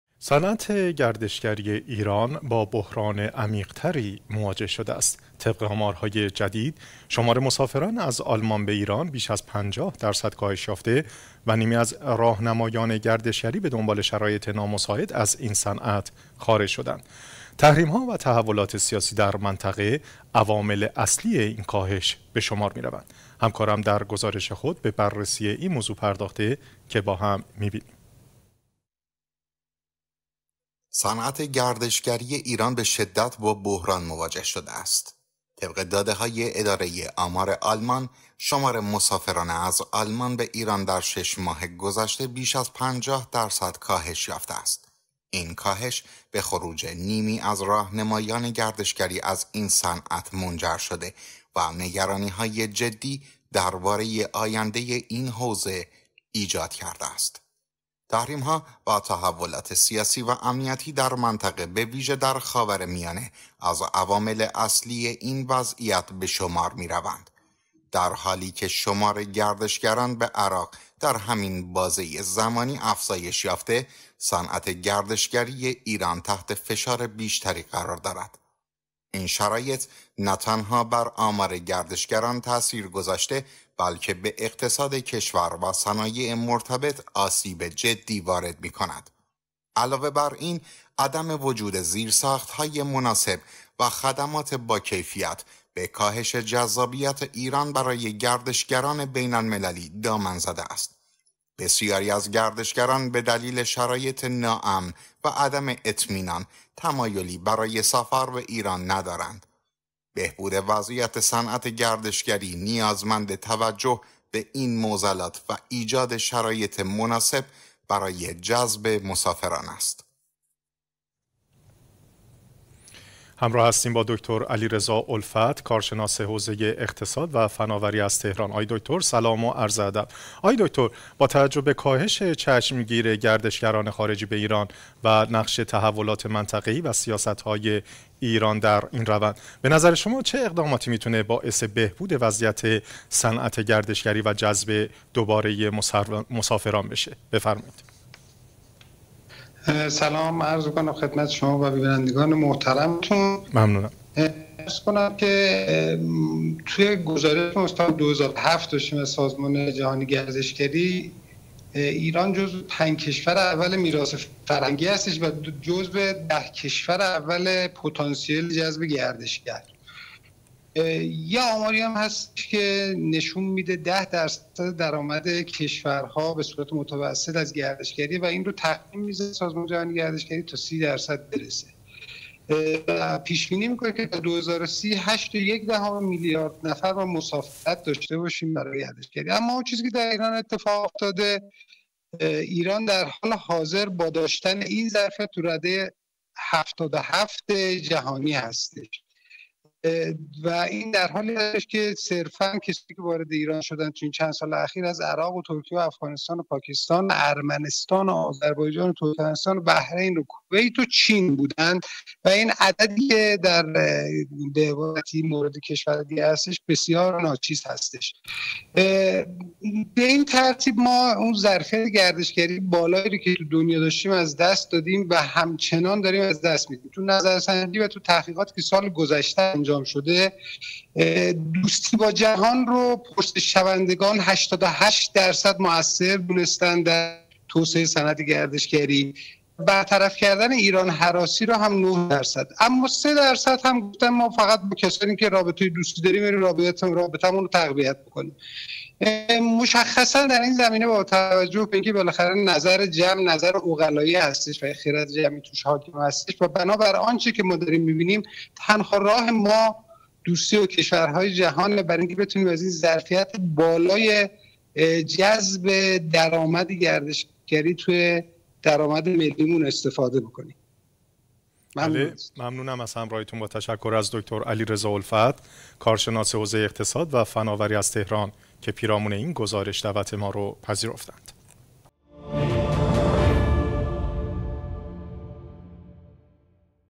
بحران صنعت گردشگری ایران: سقوط ۵۰ درصدی گردشگران خارجی؛
بحران صنعت گردشگری ایران: صنعت گردشگری ایران در حال تجربه یکی از شدیدترین بحران‌های خود است. با کاهش چشمگیر تعداد مسافران خارجی به دلیل تحولات منطقه‌ای و تحریم‌ها، بسیاری از فعالان این حوزه با مشکلات جدی مواجه شده‌اند.
آمارها نشان می‌دهد که بیش از نیمی از راهنمایان گردشگری از این صنعت خارج شده‌اند و کشورهای همسایه همچون عراق، توانسته‌اند سهم بیشتری از گردشگران را جذب کنند.

اما آیا صنعت گردشگری ایران می‌تواند دوباره شکوفا شود؟ چه تغییراتی می‌تواند موجب بهبود وضعیت فعلی و رفع بحران صنعت گردشگری ایران شود؟ آیا این مشکلات تنها به مسائل سیاسی و امنیتی مربوط می‌شوند یا دلایل دیگری نیز در کار است؟
برای پاسخ به این سوالات و بررسی کامل‌تر این بحران، توصیه می‌کنیم ویدیو را تماشا کنید تا دیدگاهی دقیق‌تر پیدا کنید.

00:00 - آغاز بحران گردشگری ایران و کاهش مسافران از آلمان
00:20 - تحریم‌ها و تحولات منطقه‌ای به‌عنوان عوامل کاهش
00:38 - آمار کاهش مسافران و خروج راهنمایان گردشگری از صنعت
01:26 - تأثیر منفی عدم زیرساخت‌های مناسب بر گردشگری
02:03 - آغاز گفتگو با دکتر علیرضا الفت درباره وضعیت گردشگری
02:21 - سوال در مورد اقداماتی برای بهبود و جذب مسافران
03:00 - تحلیل آمار گردشگری و جایگاه ایران در سطح جهانی
03:38 - از دست دادن ظرفیت بالای گردشگری ایران
ویدیوهای مرتبط

ویدیوهای مرتبط